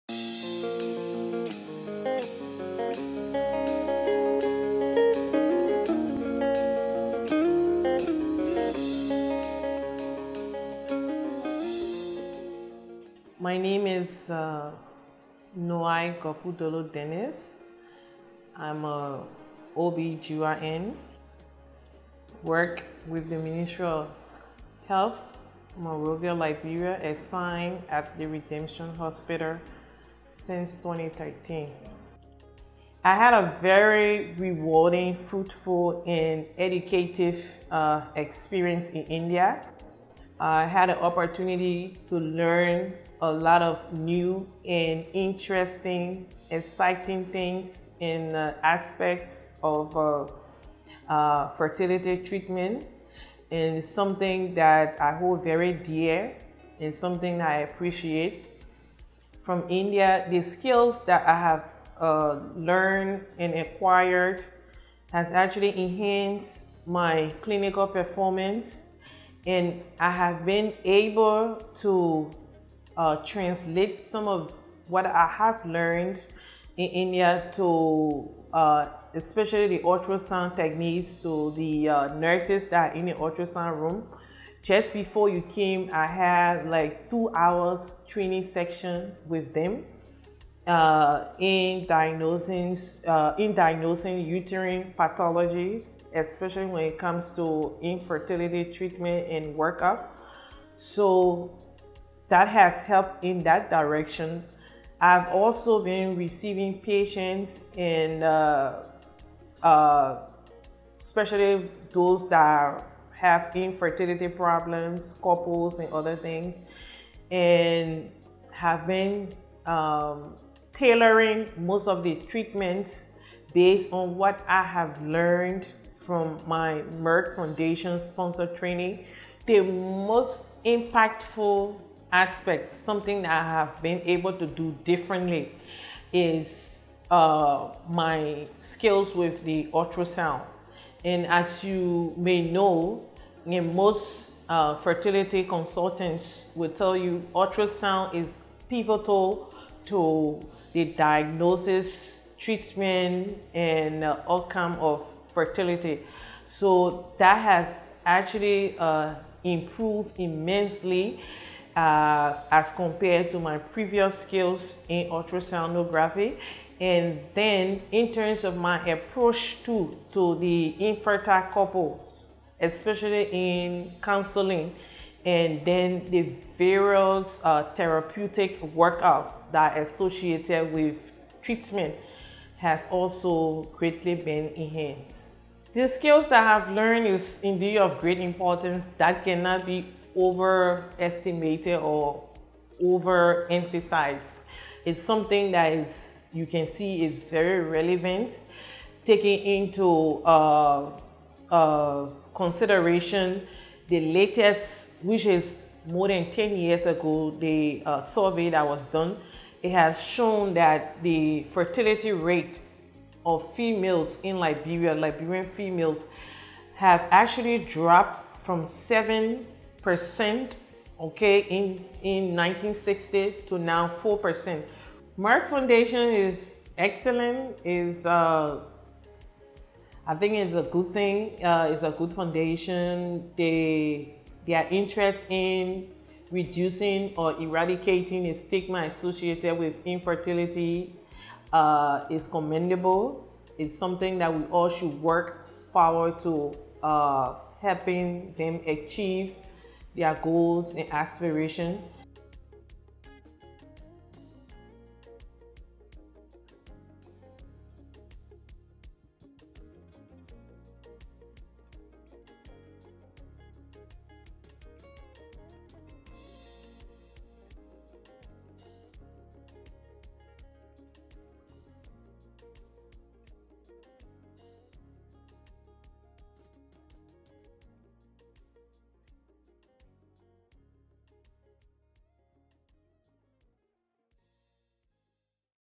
Merck Foundation Alumni, Dr. Nowiah Gorpudolo-Dennis, Fertility Specialist from Liberia
This video content is not intended for USA/Canada Audience.

Merck Foundation is making history in Liberia by supporting the training of fertility specialists and embryologists which will leave a strong legacy in shaping the quality, safe and equitable fertility care landscape of Liberia and the rest of Africa.

Through More than a Mother movement, Merck Foundation is determined to empower infertile women in Africa through access to health, information and change of mindset and encourage men to speak up about infertility issues and support their wives in the journey of making happy families.